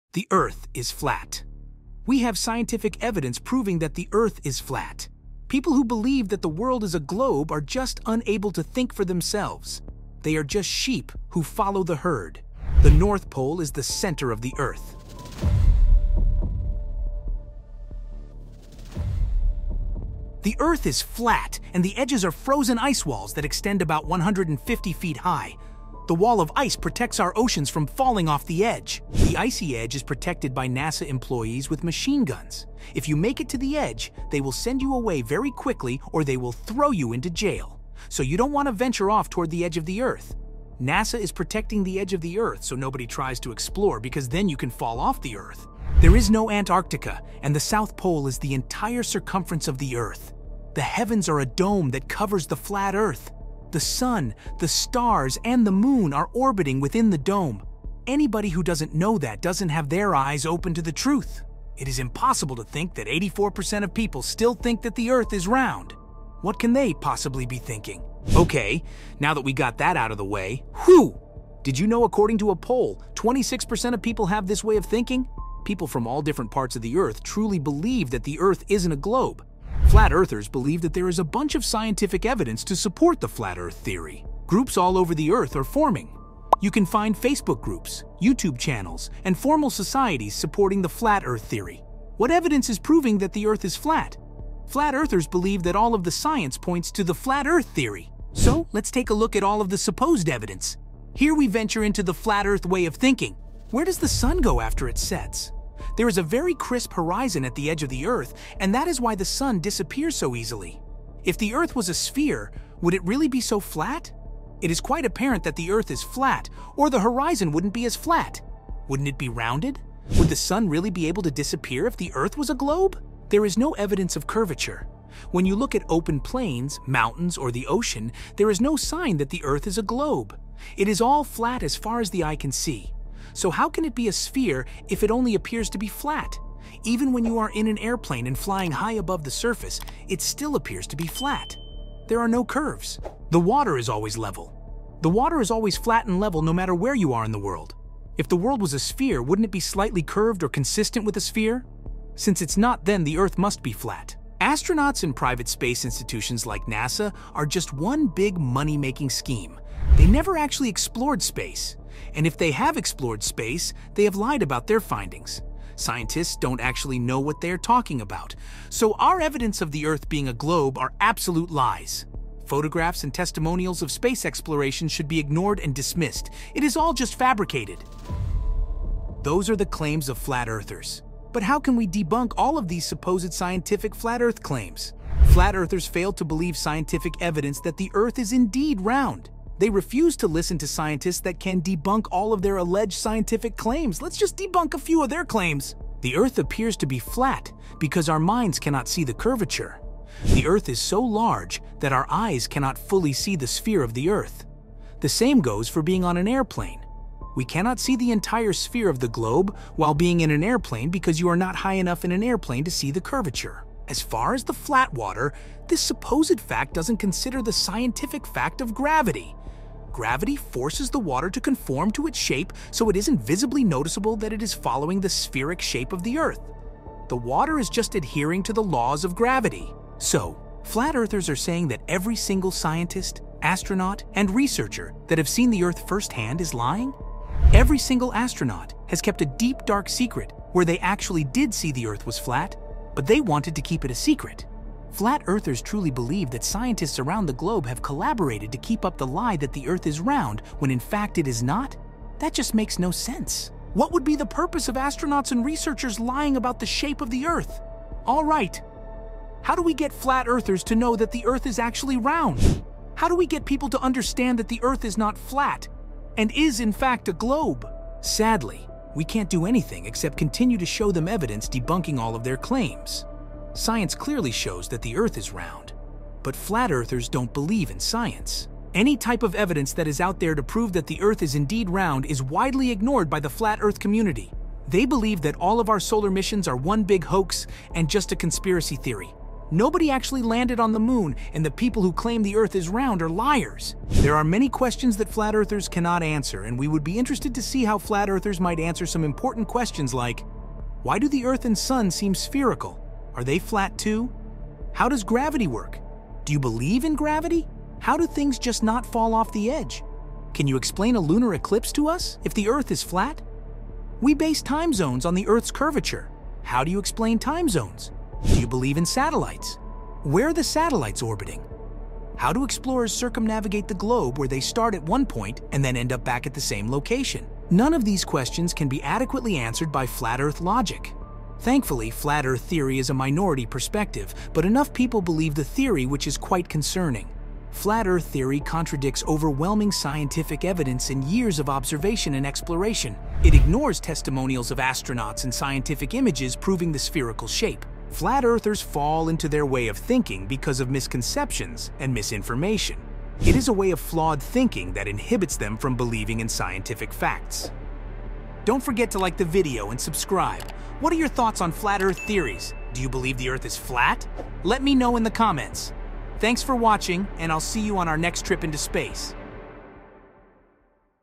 Are Flat Earthers Right?!
Explore the Flat Earth Theory in this intriguing video as we delve into its claims and scientific counterarguments. Join the conversation about the Earth's shape, examining why the sun disappears at the horizon, the concept of curvature, and more. Discover how the belief in a flat Earth clashes with established scientific understanding and learn about the challenges it faces when addressing questions on space exploration, gravity, and navigation. This video aims to foster critical thinking and engage with the scientific process while respecting diverse perspectives.

🌍 Join the Conversation on Flat Earth: Claims, Science, and Perspectives.
🔍 Examine the Evidence: Is the Earth Flat or Spherical?
🚀 Engage in Thoughtful Dialogue: Space, Gravity, and Navigating the Globe.
🔬 Explore the Intersection of Belief and Science.